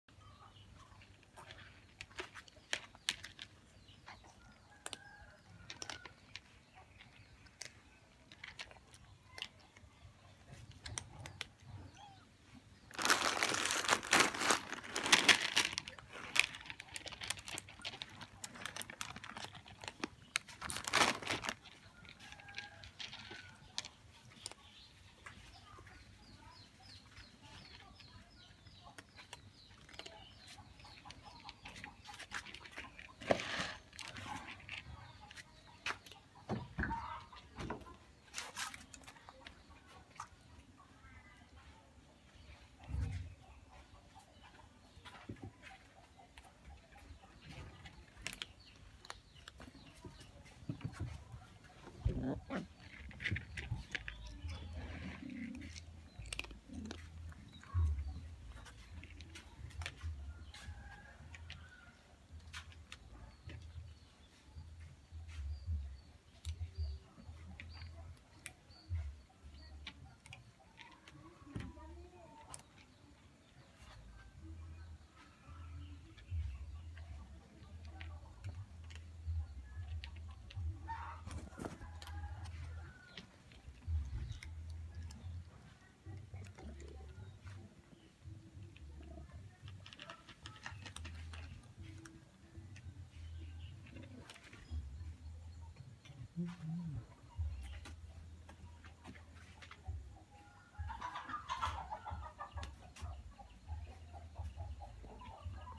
ada 5 kucing1
kucing ini datang sendiri ke rumah,dan saya pelihara